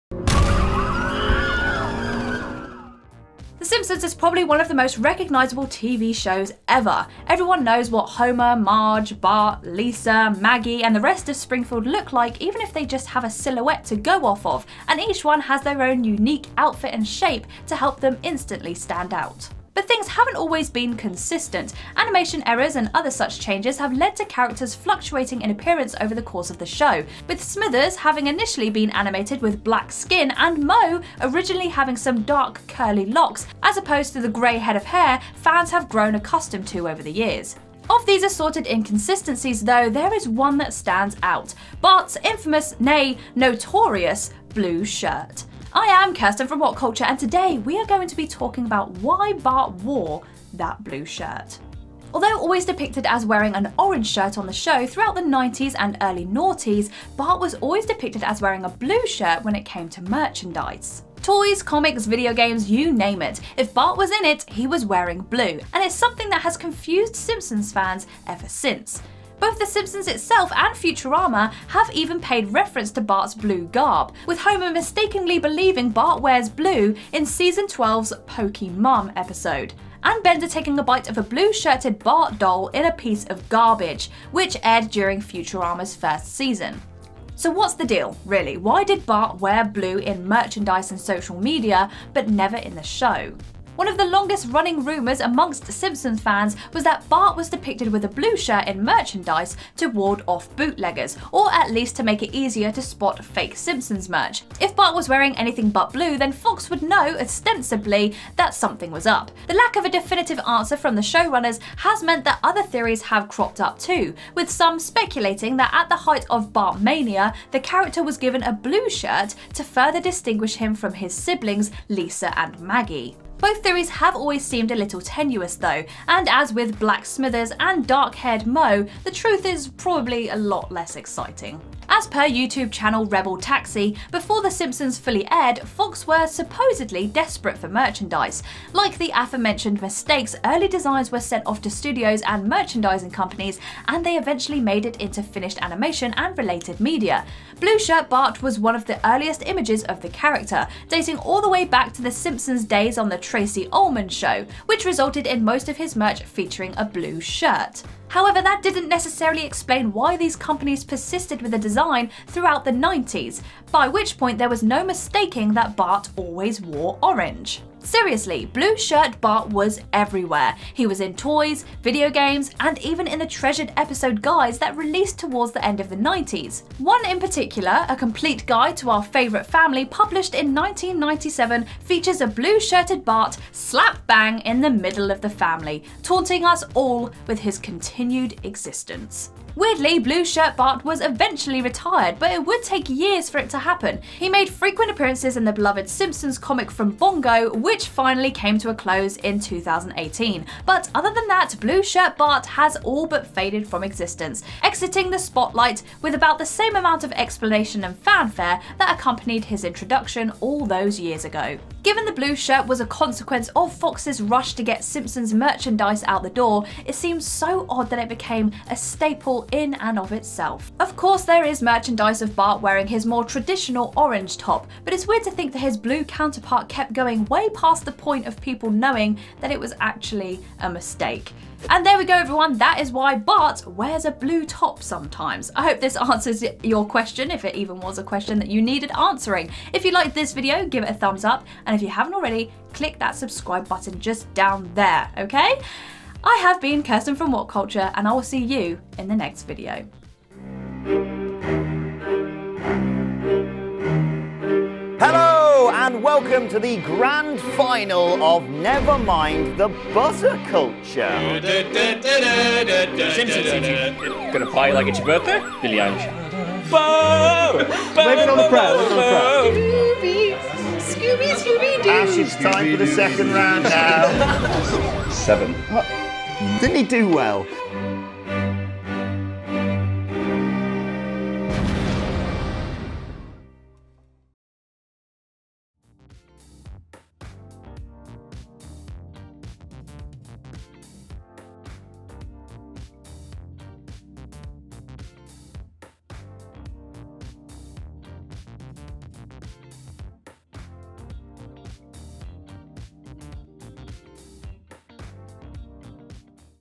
The Real Reason Behind Bart Simpson's Blue Shirt
Ever wondered why Simpsons merch always had Bart in blue?